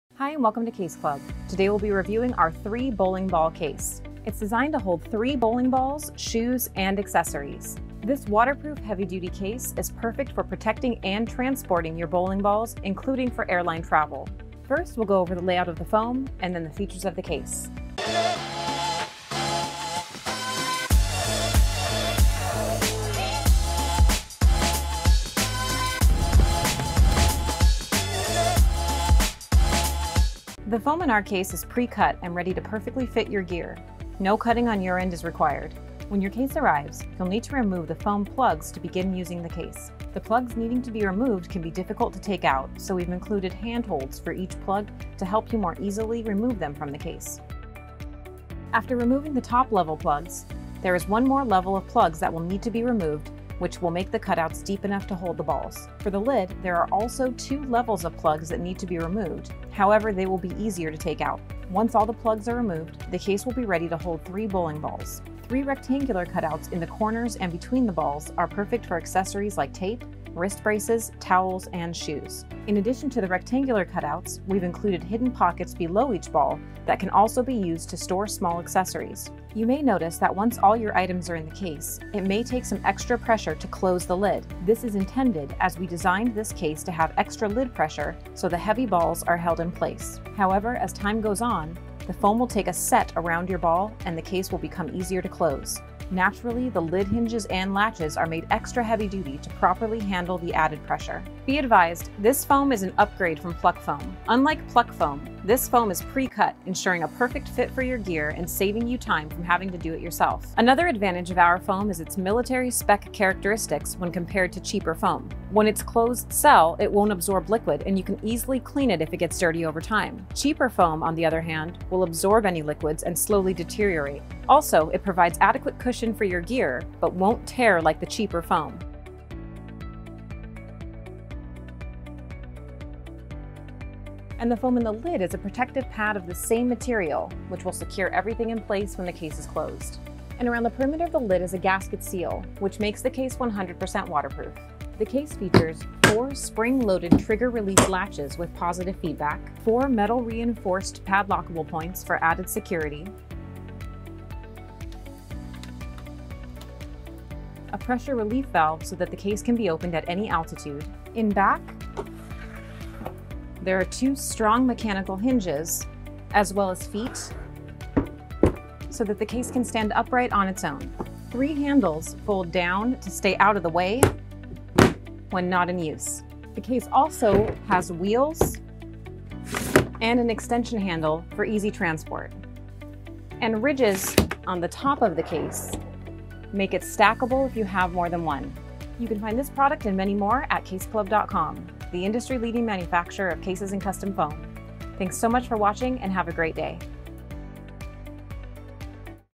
3 Bowling Ball Case
Description
Case Club’s Triple Bowling Ball Case is made to securely hold and transport 3 bowing balls, plus shoes, tape, wrist brace, towels, grip sack, and more in a heavy duty, waterproof, airtight, pressure equalizing case. For those who do not want to have their bowling balls break because of mishandling by the TSA or a shipping company, this case was built with you in mind. Even when filled to the brim with three 16 lb balls plus accessories, this case is rock solid. 2 wheels with a handy extension handle make transportation a breeze, and padlock holes allow for extra security. The foam is military grade polyethylene to make sure your bowling balls do not shift during transit or even during rough hits and drops. Protect your investment, buy Case Club!

Category
Science & Technology